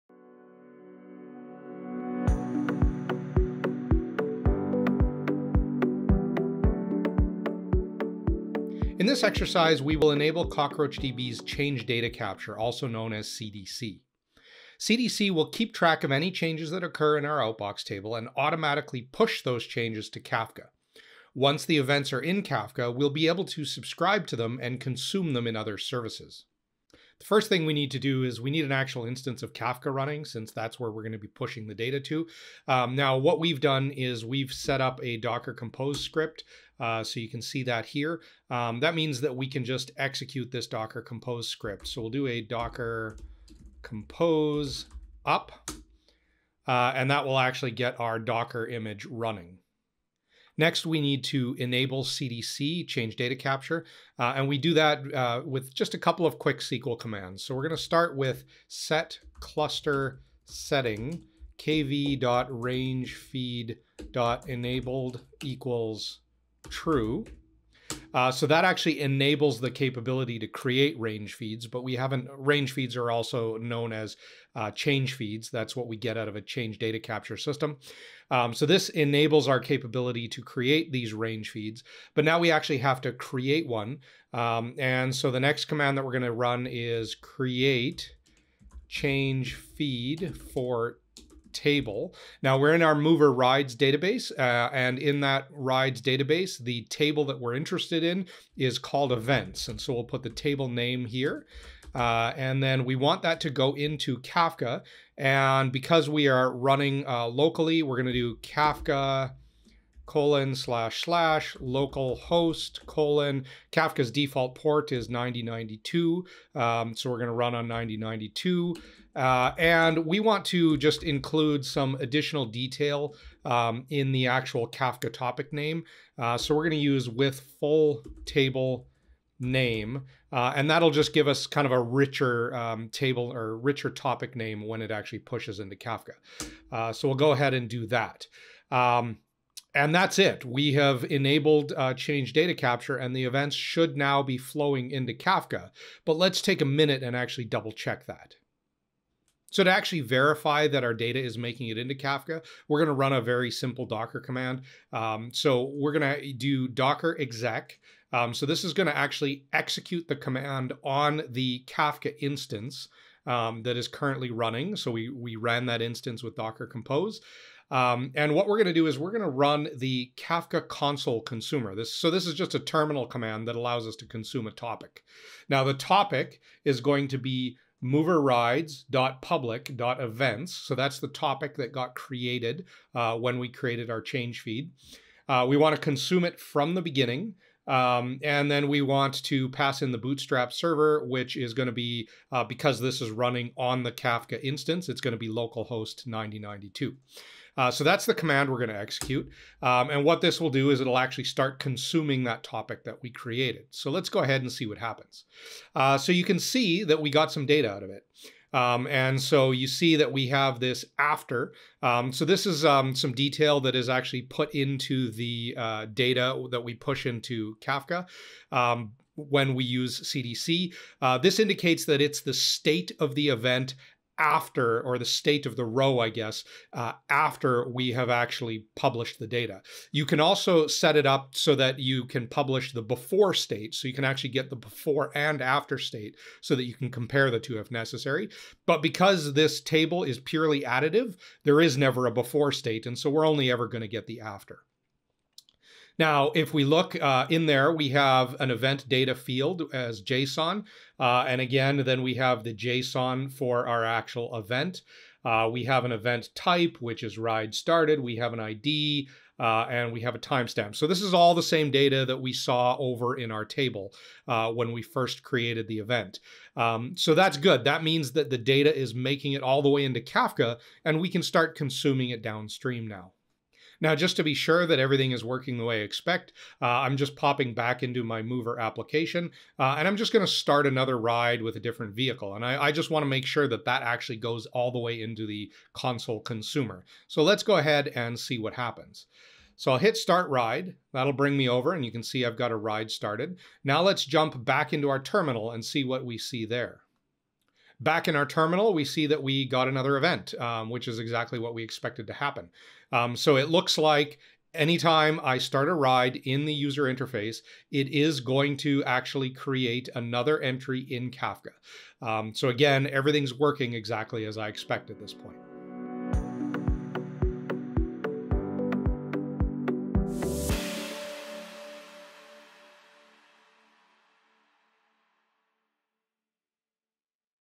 Enable Change Data Capture
This is an exercise from the Cockroach University course “Event-Driven Architecture with Java”.

In this exercise, students will be setting up Cockroach CDC to consume events from an Outbox Table in CockroachDB and push those events to Kafka.

They will enable Change Feeds in CockroachDB.
They will create a Change Feed for the Outbox Table.
They will verify that when an event is added to the Outbox Table it is published to a Kafka topic.

Learn more about CockroachDB by signing up for free training at Cockroach University

00:00 How do we enable change data capture?